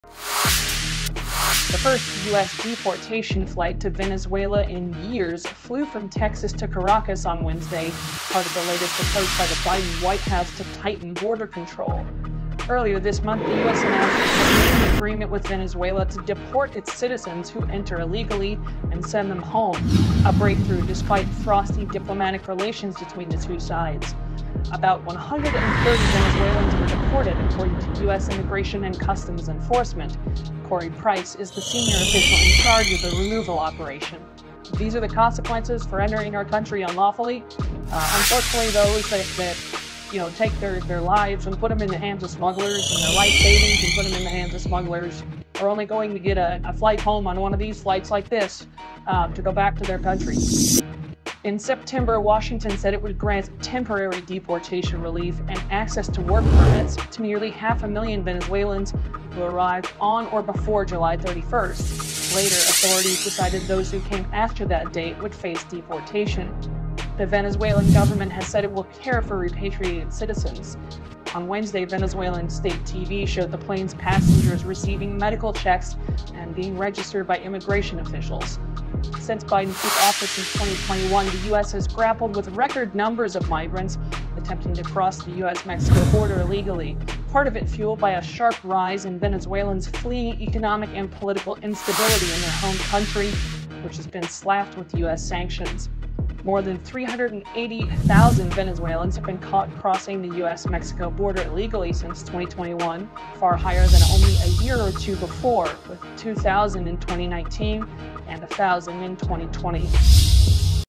First US deportation flight in years lands in Venezuela
Deportation,
Venezuela,
US,
Biden,
Migrants

💗📌 Singapore